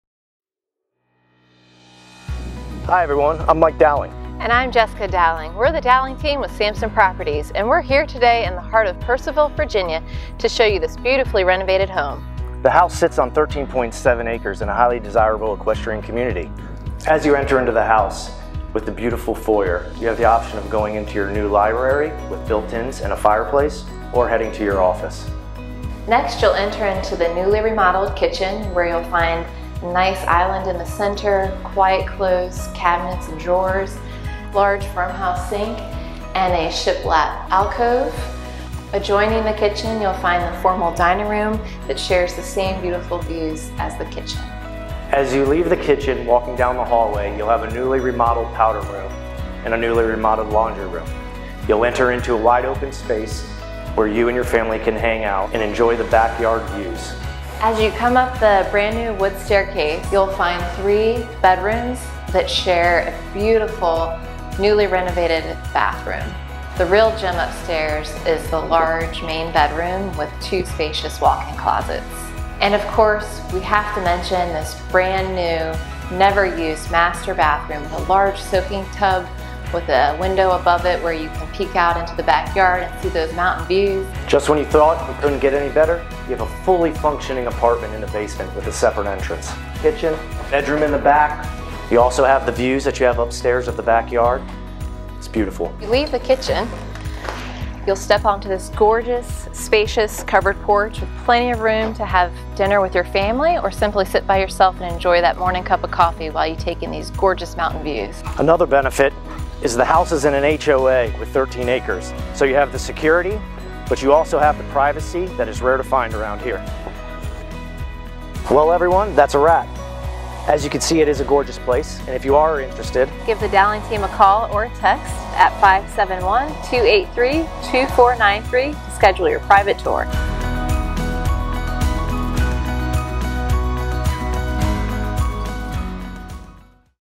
37890 Campbell Clan Lane - Purcellville, VA
Tour an unbelievable gem in the heart of a highly sought-after equestrian community in Purcellville, VA.